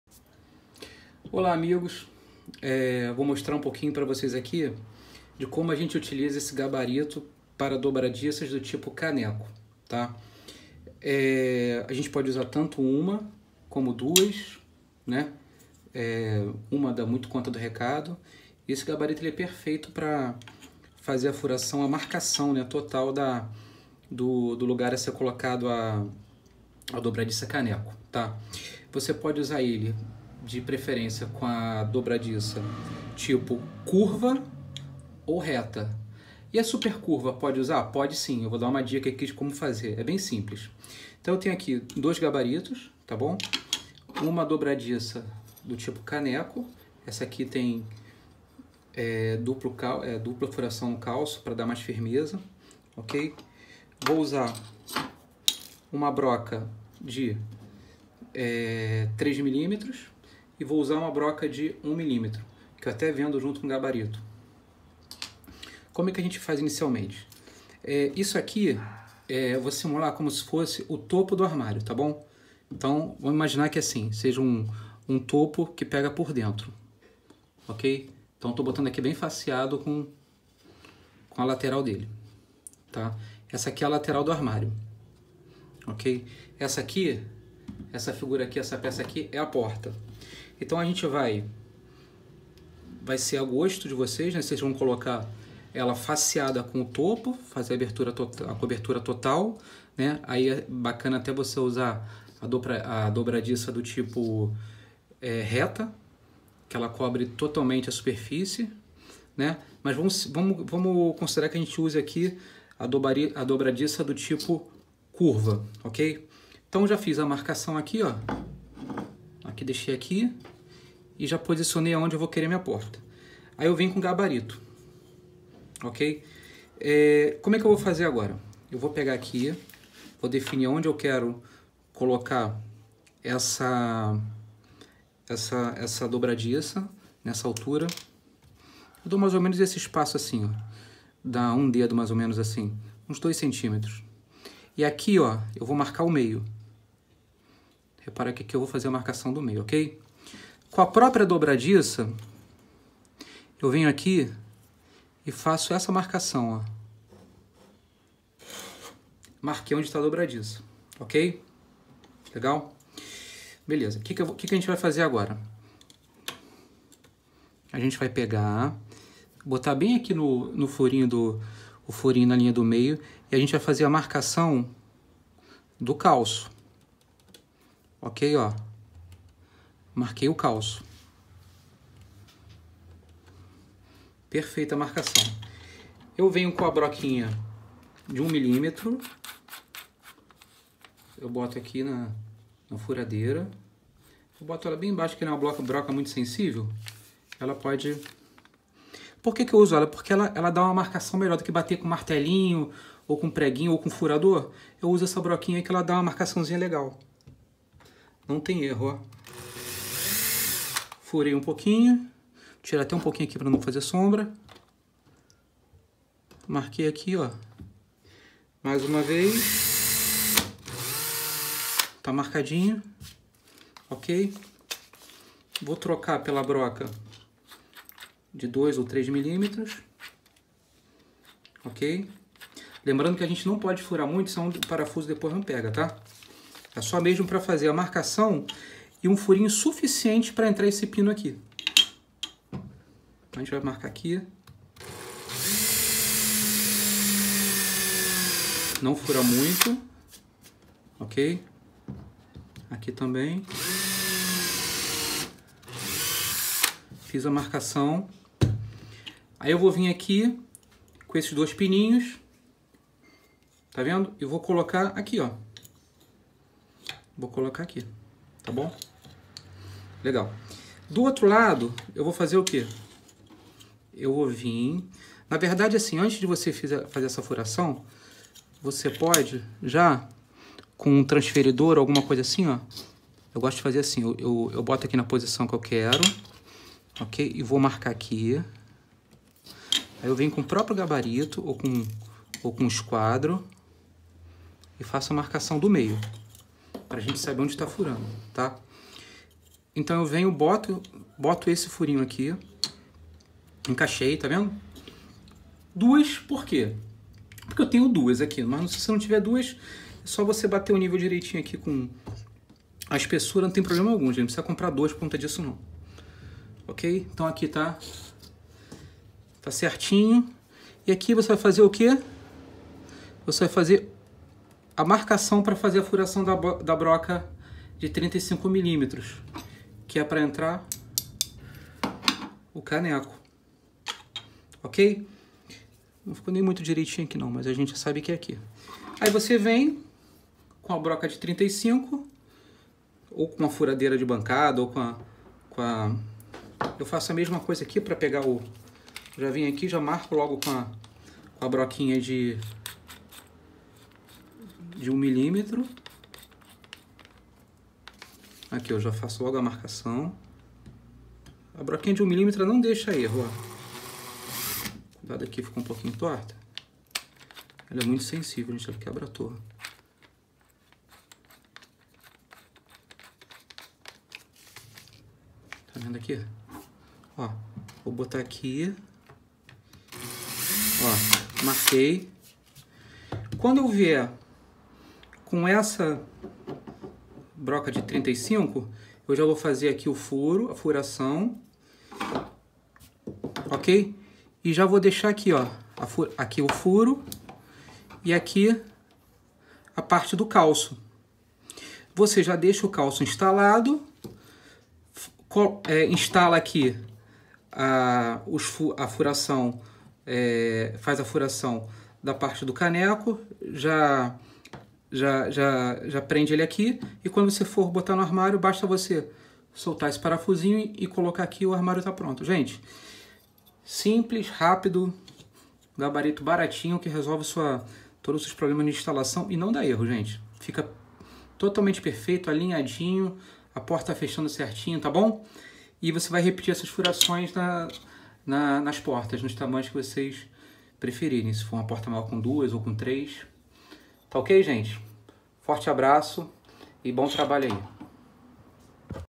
Gabarito para dobradiças tipo Caneco
Como instalar portas de armário com um gabarito para dobradiças